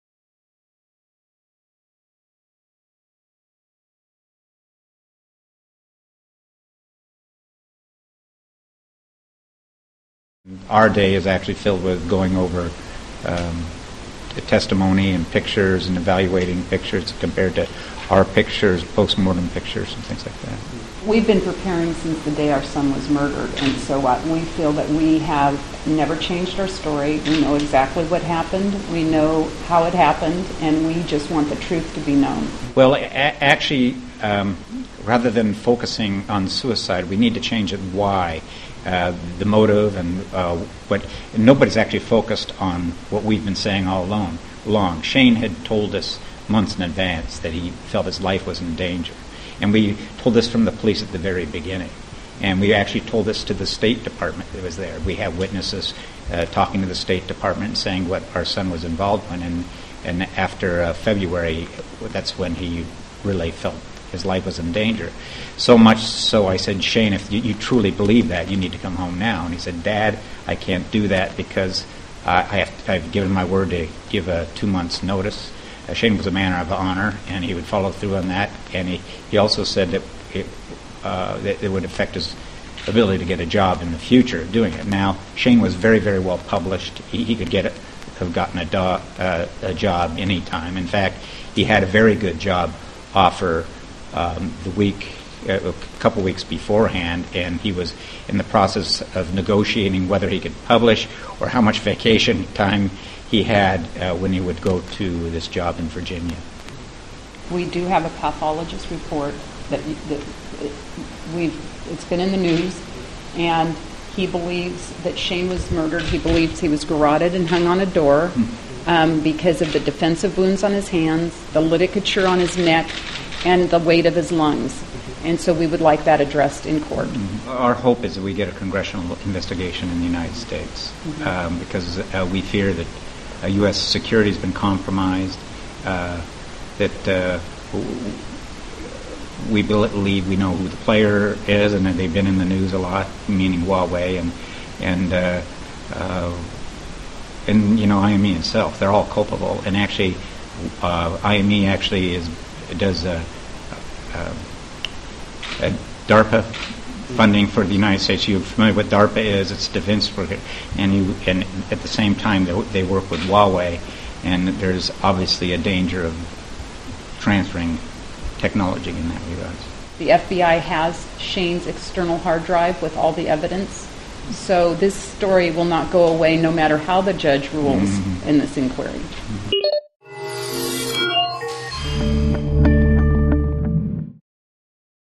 Shane Todd's parents call for US congressional investigation
The parents of American researcher Shane Todd want a United States congressional investigation into his death.  Rick and Mary Todd, who believe their son was murdered in connection with his work, are in Singapore ahead of a coroner's inquiry into their son's death.